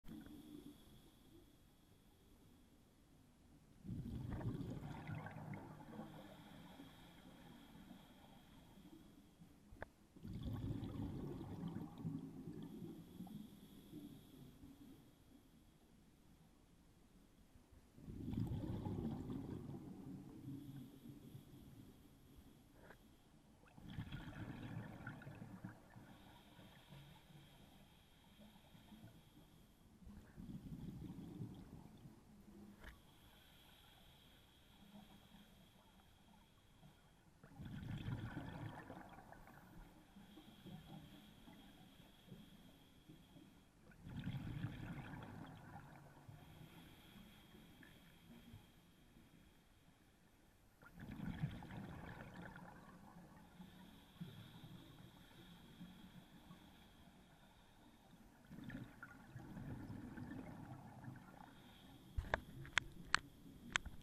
Quillback Rockfish and Painted Greenling
First dive with my SJ5000x at Tyee Cove in Nanoose, BC